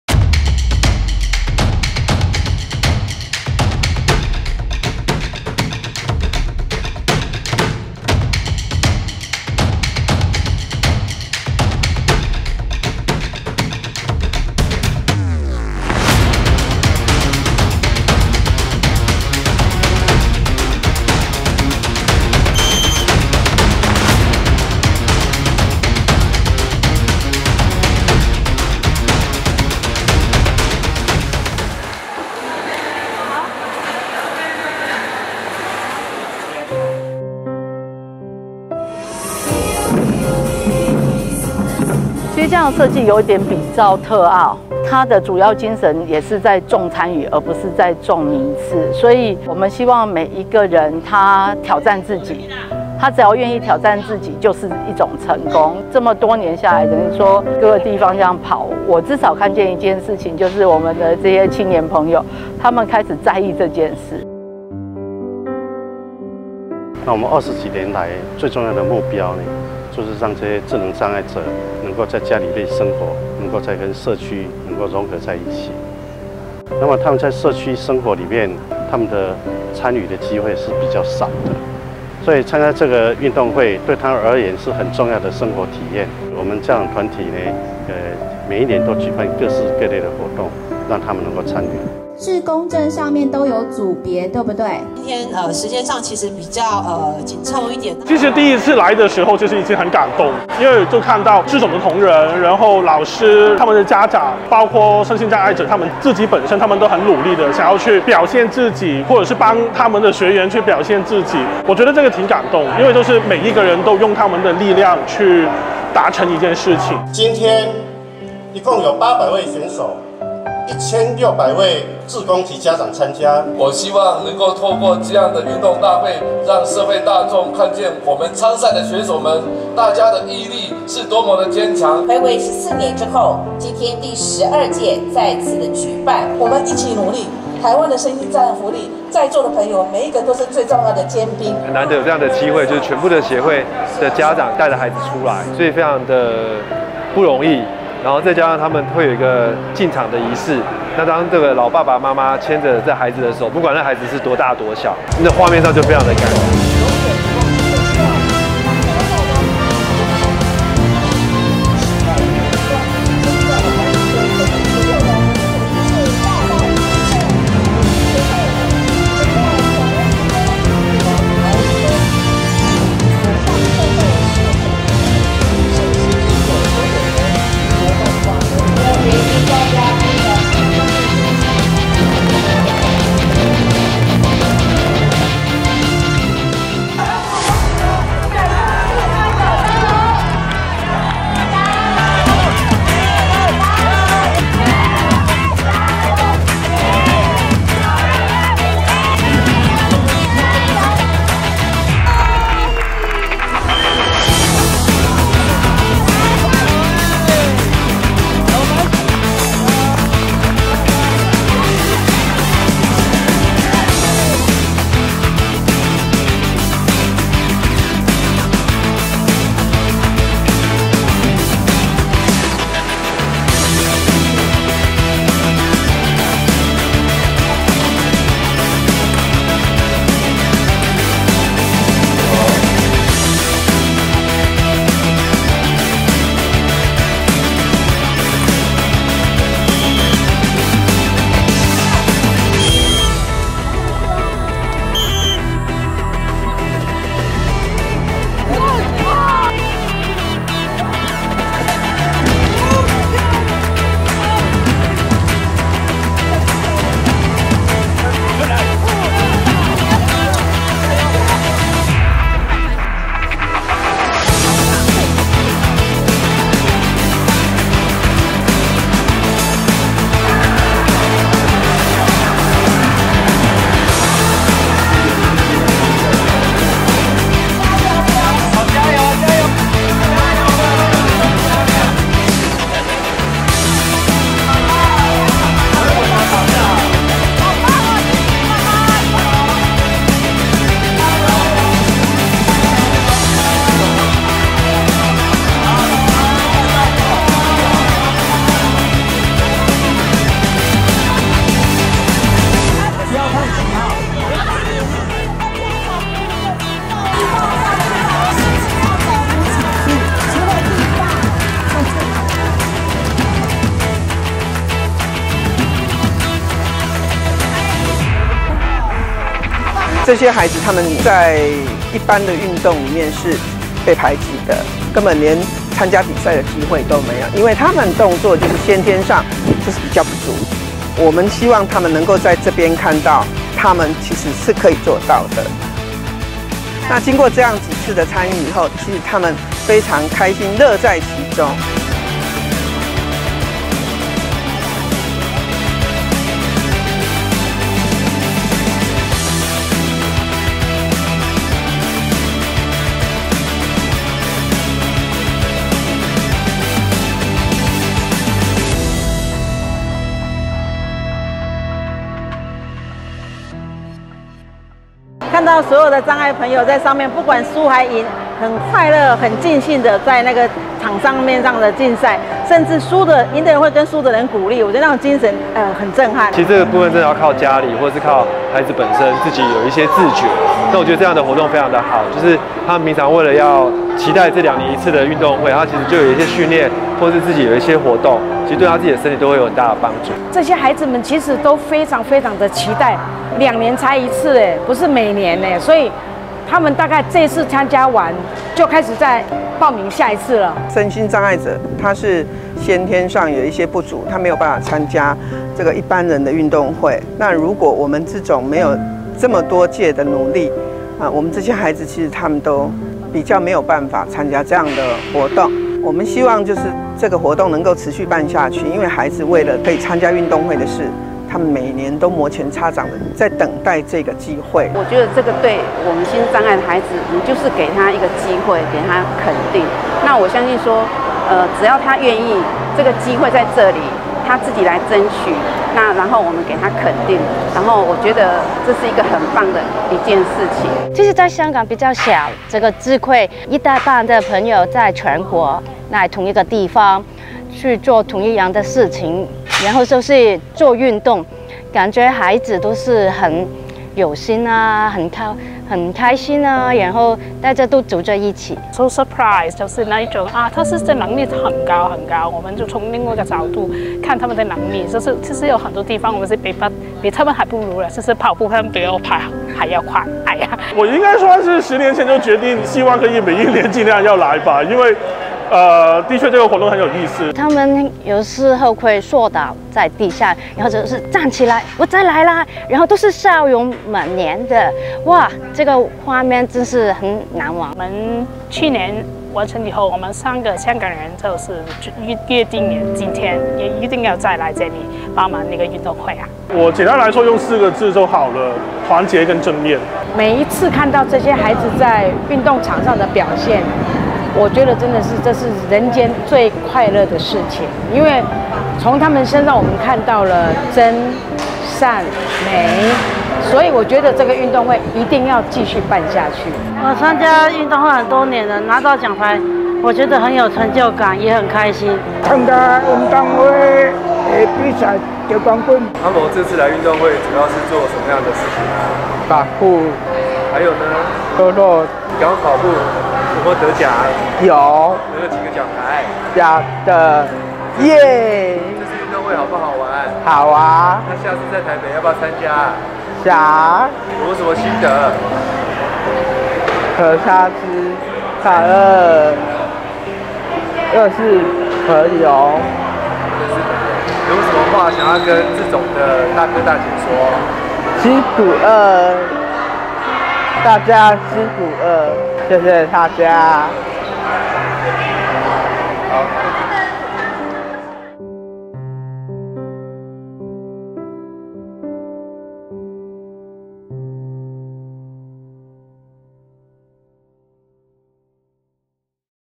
【活動花絮】2019第12屆全國心智障礙者親子運動會在台中_智障者家長總會｜觀賞故事
本屆運動會近有800位智能障礙運動員，加上家長及志工、工作人員等兩天共計超過2,500人次參加。

@智障者家長總會 深信，運動會的舉辦不僅是一場活動，更是彰顯心智障礙者也有權利與他人一樣，在運動場上挑戰自我，享受運動競賽的樂趣！

連續四屆自費來台的香港志工說：「其實第一次來的時候，就已經很感動了。因為我看到智總的同仁、各參加單位的老師、家長，包括運動員自己都很想要努力的去表現自己，或者是幫他們的學員去表現自己。我覺得挺感動的，因為每一個人都是用他們的力量，去達成一件事情！」

為了你們的笑容，運動會一定要一直辦下去！